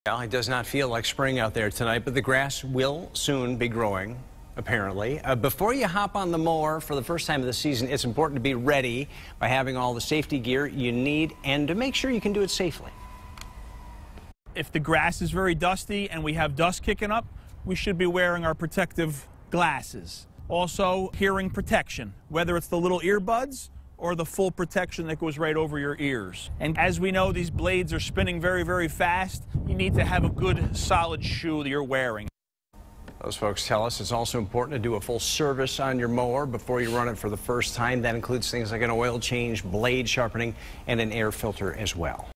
Lawn care safety tips
It might not seem like spring yet, but the grass will be growing soon enough. Before you hop on the mower for the first time this season, it's important to be prepared by having all of the safety gear needed to do it safely.